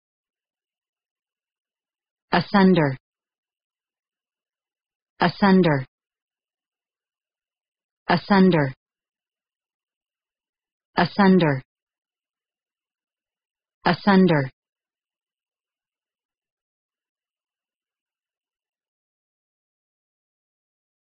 How to pronounce asunder
asunder American English pronunciation. How to pronounce asunder correctly. How to say asunder in proper American English.